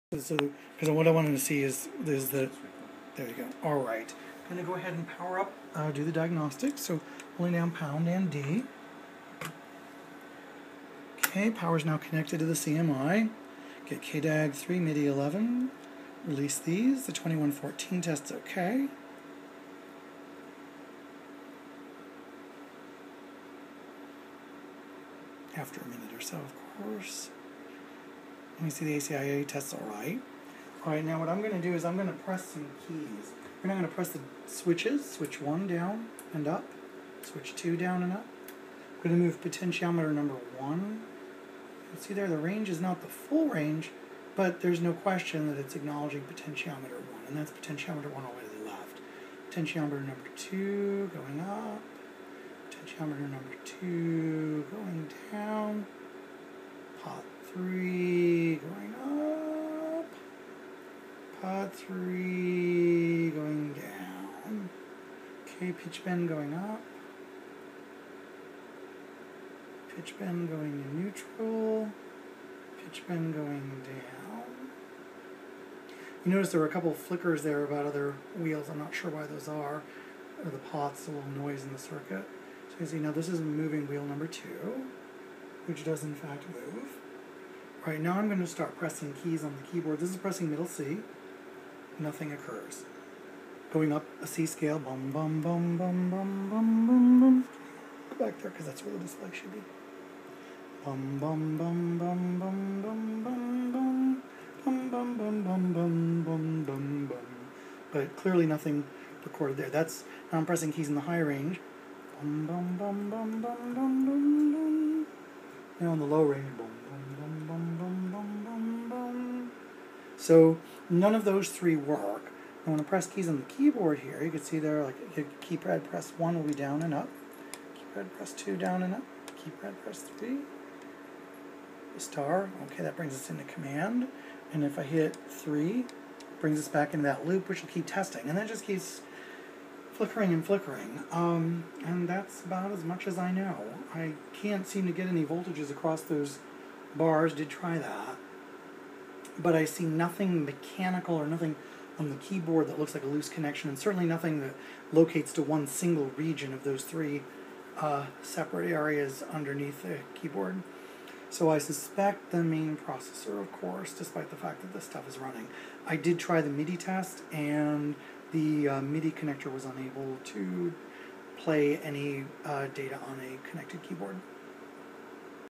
Fairlight CMI Keyboard Tests
Me running the diagnostics, demonstrating what works and what doesn't.